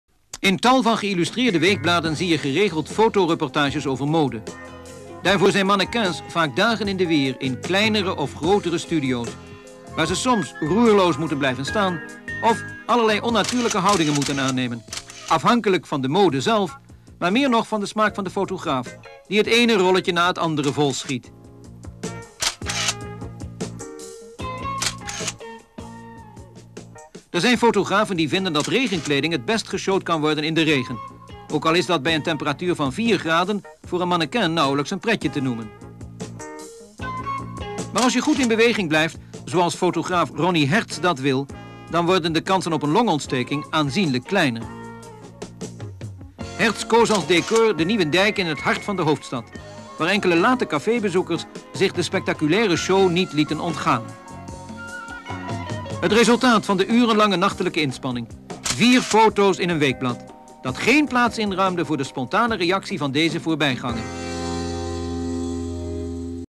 How is a fashion photo reportage made? (Week number: 78-17) 🔴
Description (more): Focus on the making of a photo reportage on fashion. SHOTS: model during a photo shoot in a studio; models show rainwear at the Nieuwe Dijk in Amsterdam for a photo reportage by Ronnie Herz.